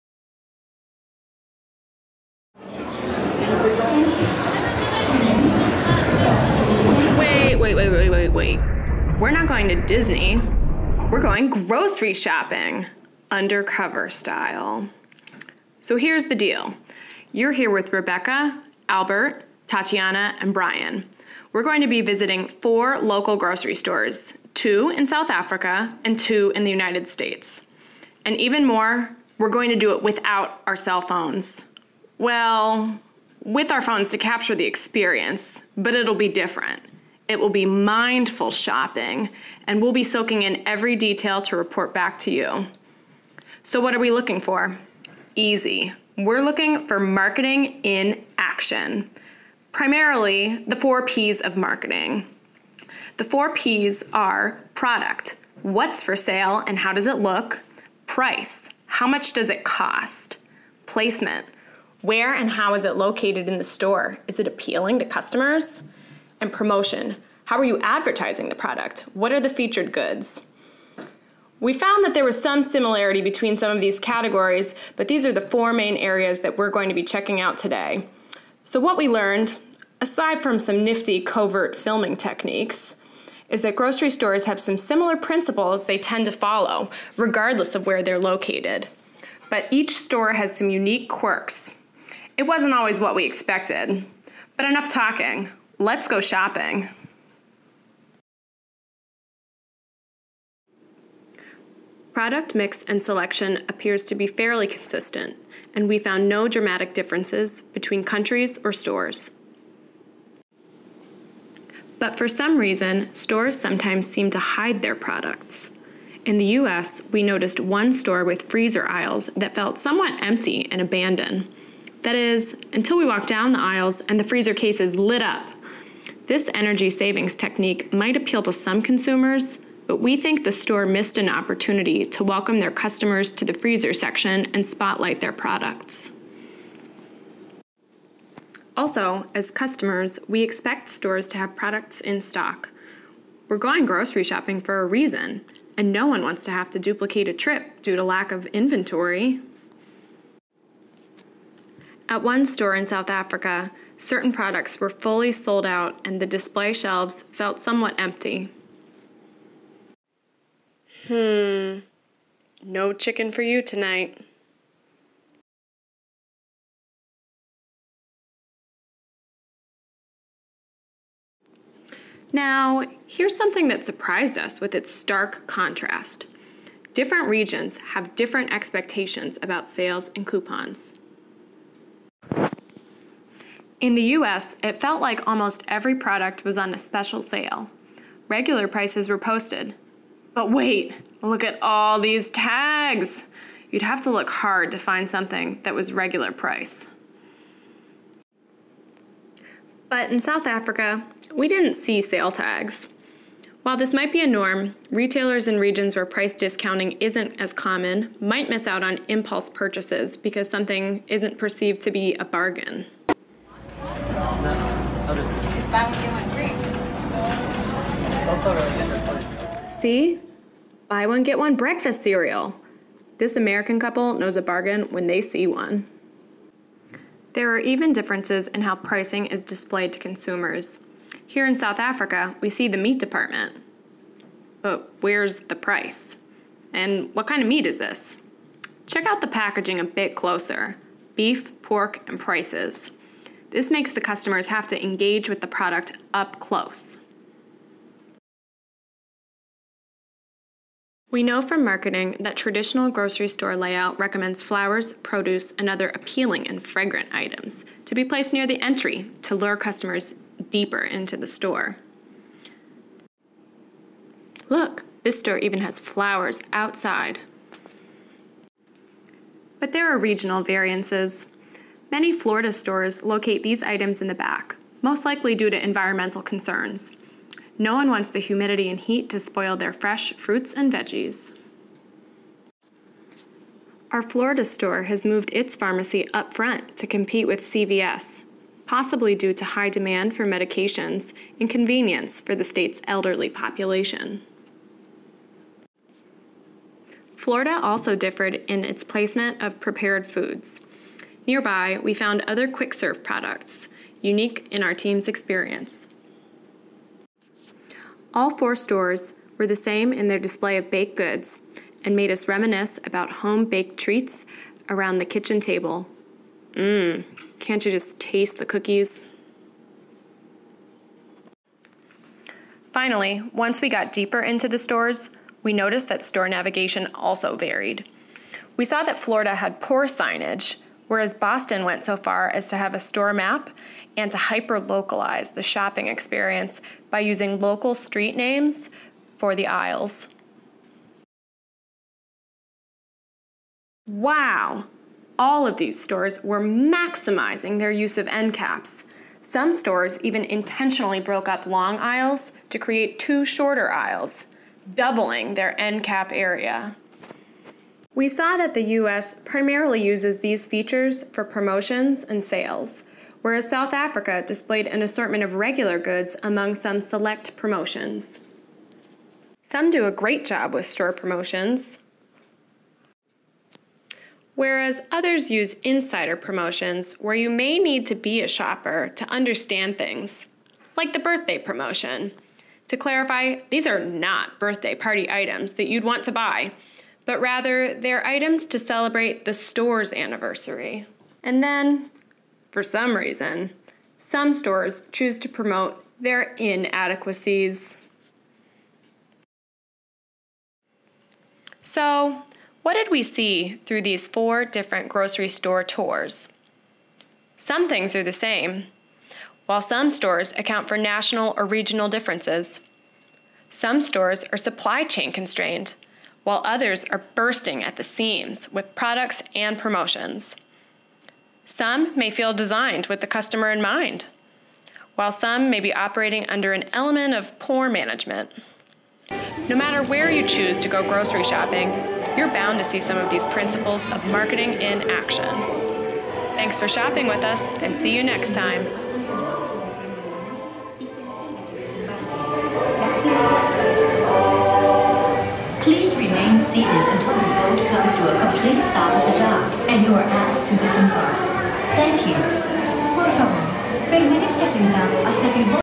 Grocery Store Tour
Check out four grocery stores and the shopping experience in supermarkets across the U.S. and South Africa. MarketingInAction 4Ps MGMT-S6000